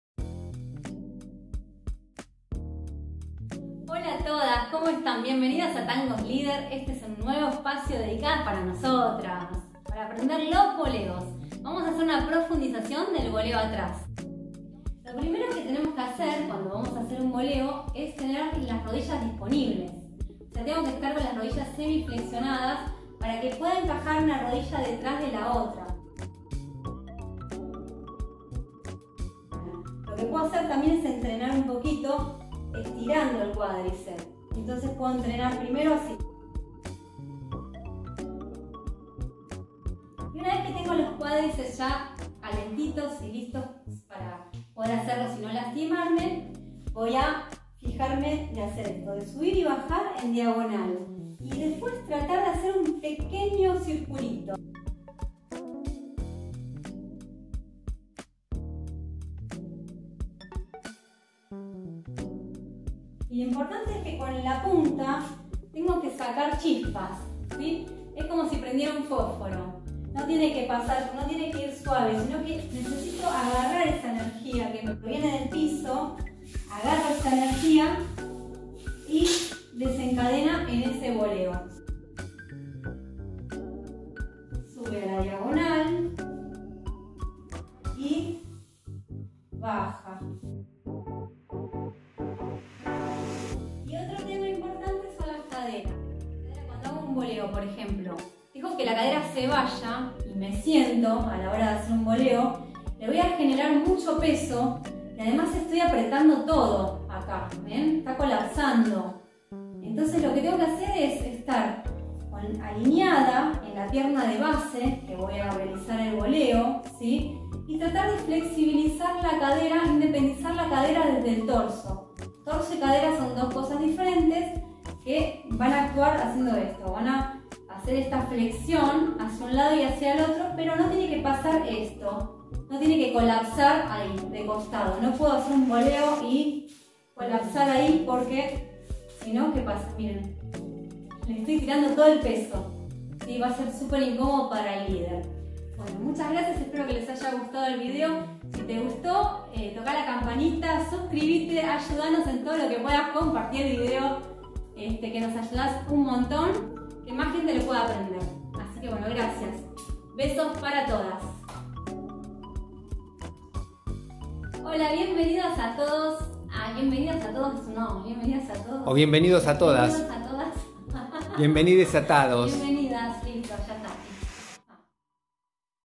Boleo Atrás, técnica femenina por Noelia Soldera
Hola a todas y todos!!
En éste video veremos el paso a paso del Boleo atrás , técnica y tips!!! Deseamos que sea de utilidad éste video!! abrazotes!! 🥰🤗

 Cómo DONAR?
Contribución voluntaria en Argentina:
BONO de pesos ars. $600
BONO de pesos ars. $400

TanGostLeader
Damian Esell y Noelia Soldera 🌈